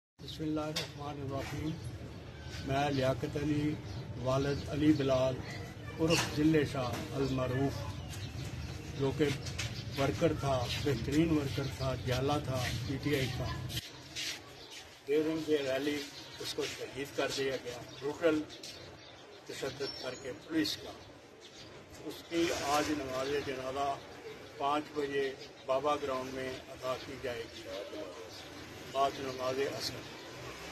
پولیس تشدد سے شہید ہونے والے پی ٹی آئی کارکن علی بلال ظل شاہ کا نمازجنازہ سیکرٹریٹ کے پیچھے
Video from ✍️®SadiQ KhaNn®✍️